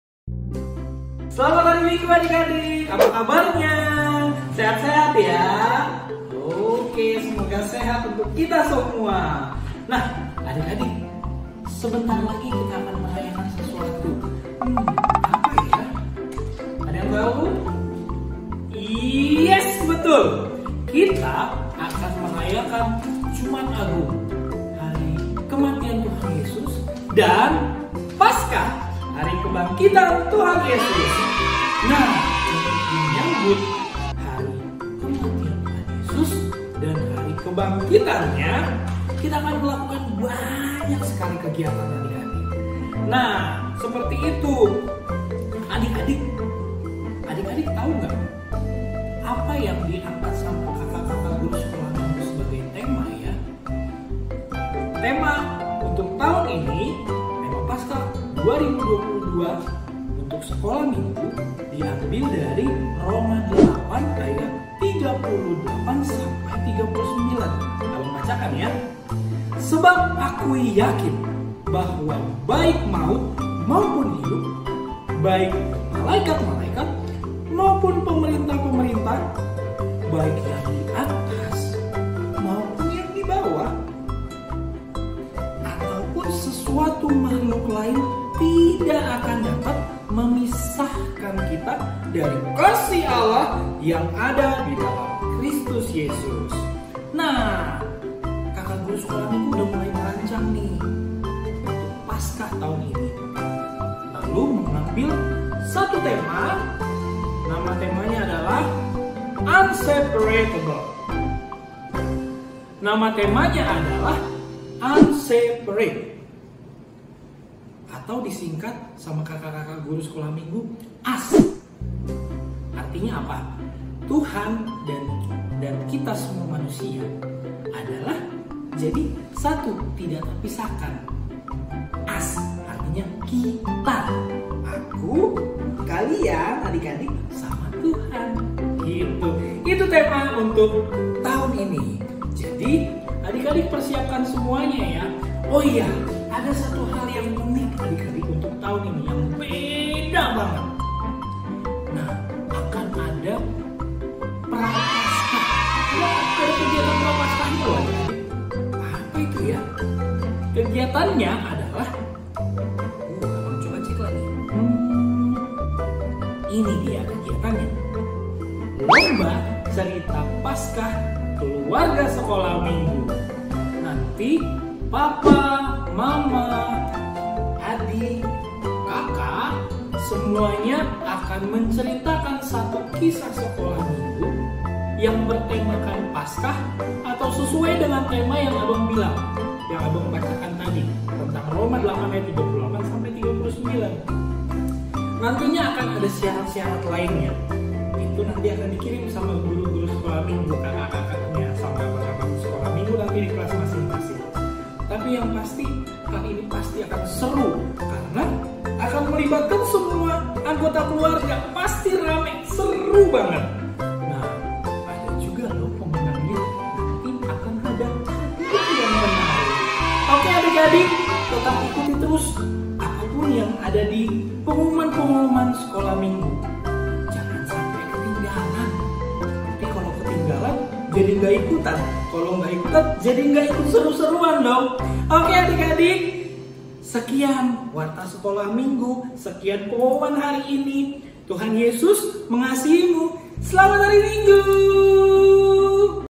Warta Paskah 1
Video Pengumuman tentang Paskah Sekolah Minggu HKBP Srengseng Sawah 2022